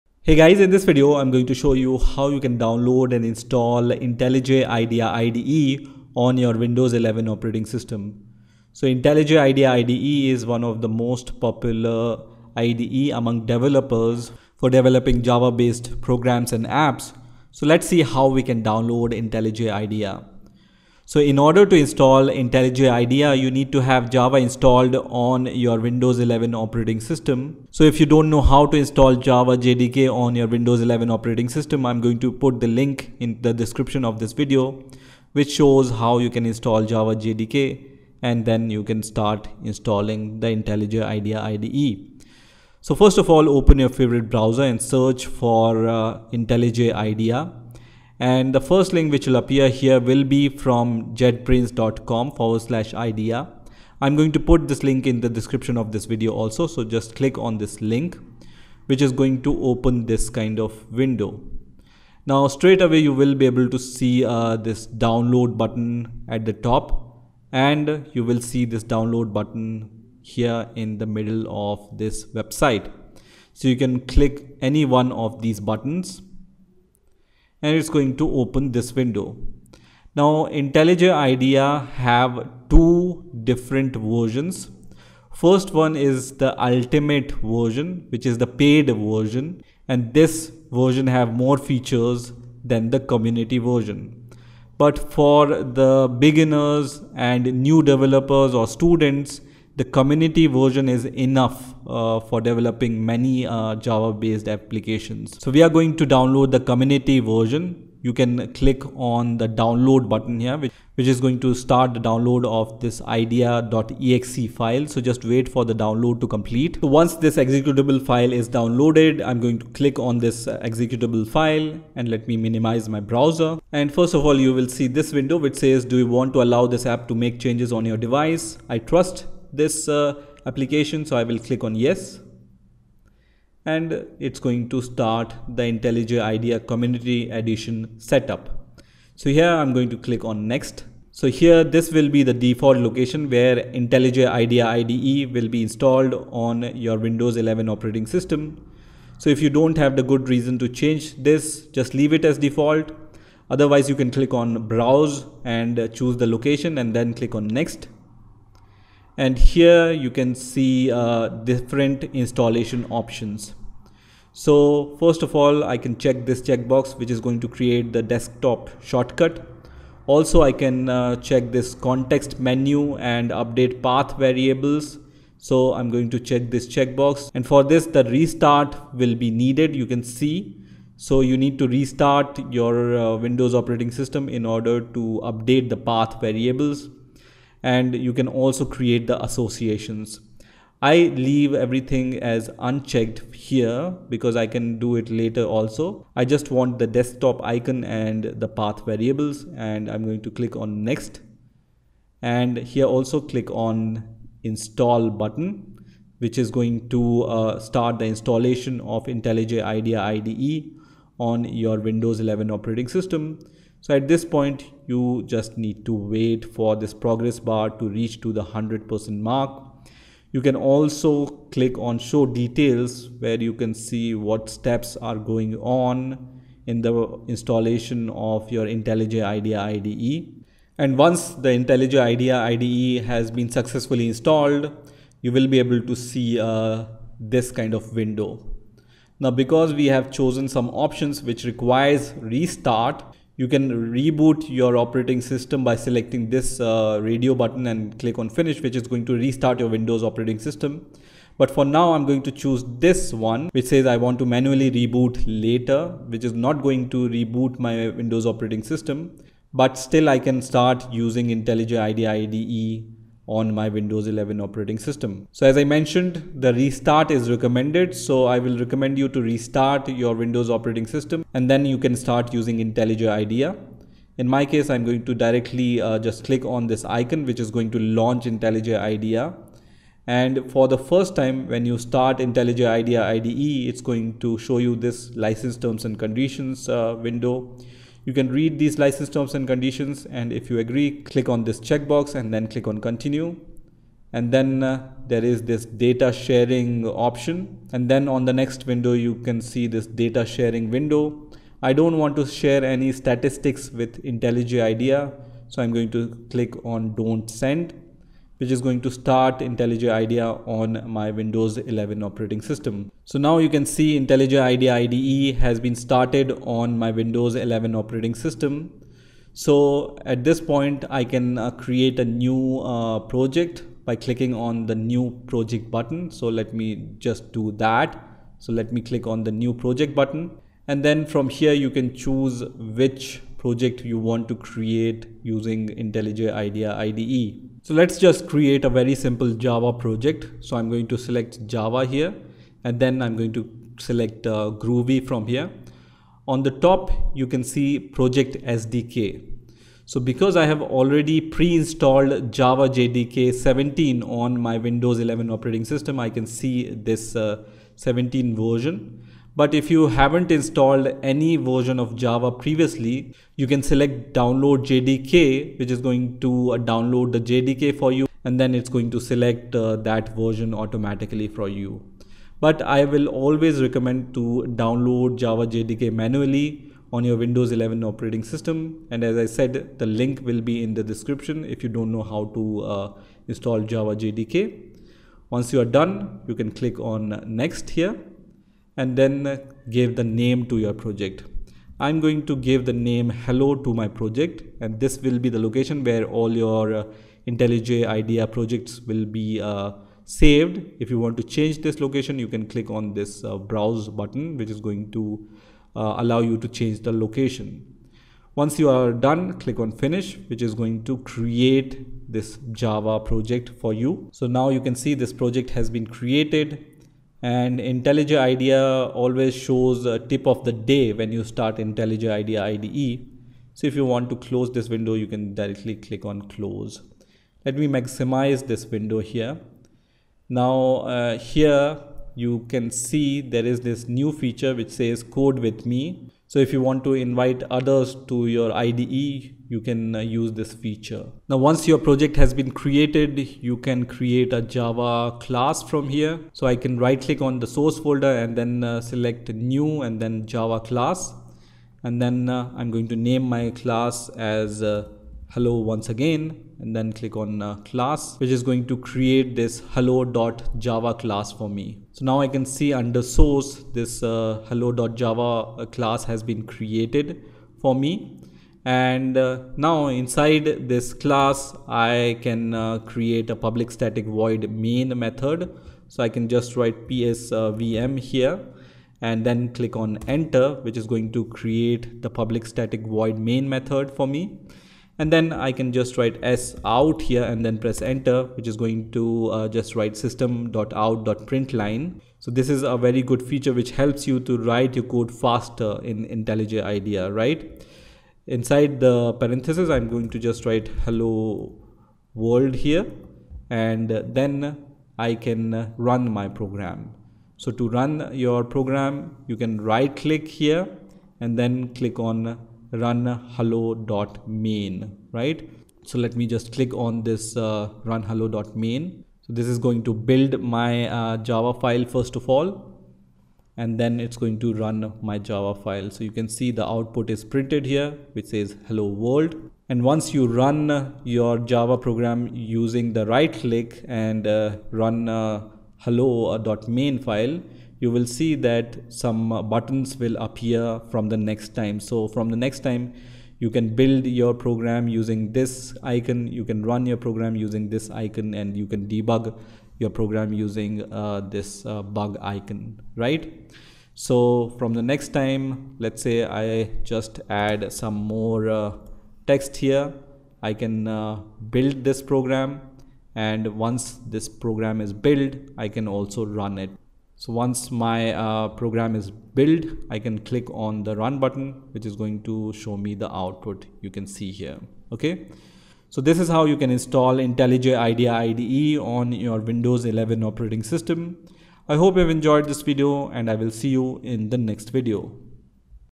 How to Create First Java Program in IntelliJ IDEA IDE
In this Video I am going to show How to Install Intellij IDEA on  Windows 11 . In addition I am also going to show How to Create First Hello World Java Application. Downloading, installing and starting IntelliJ IDEA in easy steps demonstrated.
Creating First Hello World. jetbrains intellij idea tutorial. So let us see How To Install IntelliJ IDEA for Java on Windows 11.